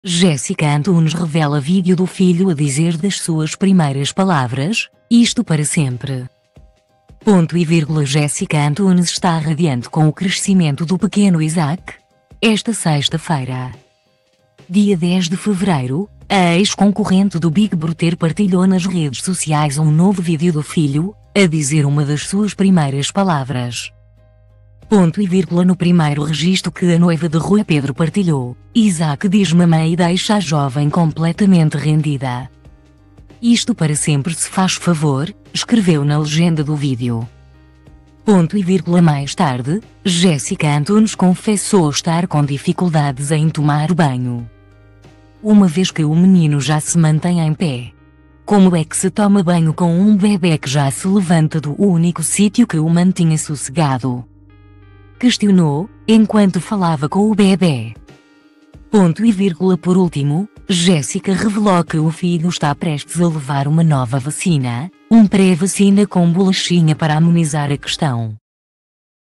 Jéssica Antunes revela vídeo do filho a dizer das suas primeiras palavras: “Isto para sempre”
;"Jéssica Antunes está radiante com o crescimento do pequeno Isaac. ;"No primeiro registo que a noiva de Rui Pedro partilhou, Isaac diz “mamã” e deixa a jovem completamente rendida.